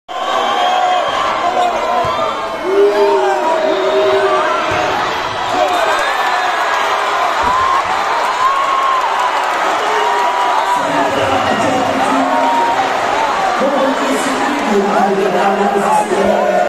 VakıfBank'ta Neslihan Demir hakeme tepki
Kupa Voley finalinde VakıfBank ile Fenerbahçe karşı karşıya gelirken son sözü sarı-siyahlı takım söyledi. Özellikle son set büyük çekişmeye sahne olurken VakıfBank'ta Neslihan Demir hakeme tepki gösterdi.
◥ Skorer'i takipte kalın...